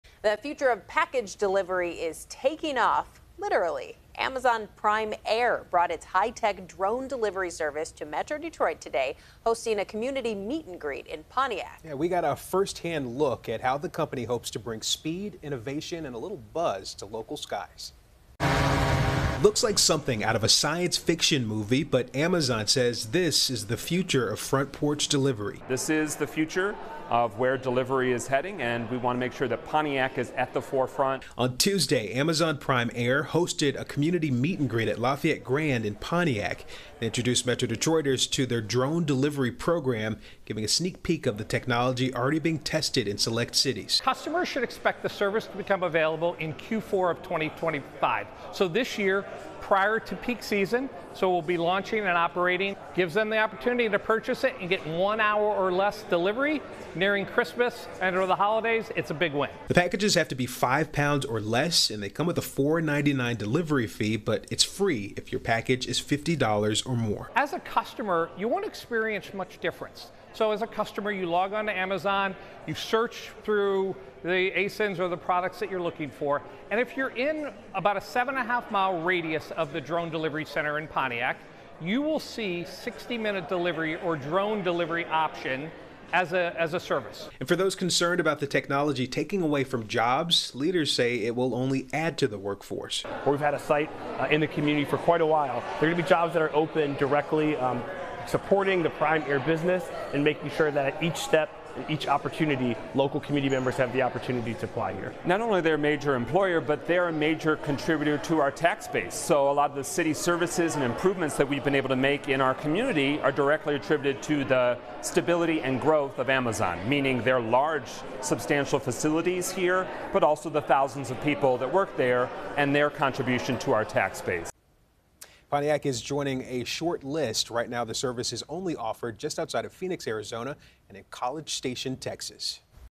Amazon gives sneak peek of its Prime Air program in Pontiac, Michigan
With drone delivery set to take off in late 2025, Amazon is betting big on Pontiac, Michigan. The tech giant's drone program, Amazon Prime Air, is setting the stage for a high-tech transformation in how packages arrive at your front door, and Pontiac is one of the first communities on the flight path.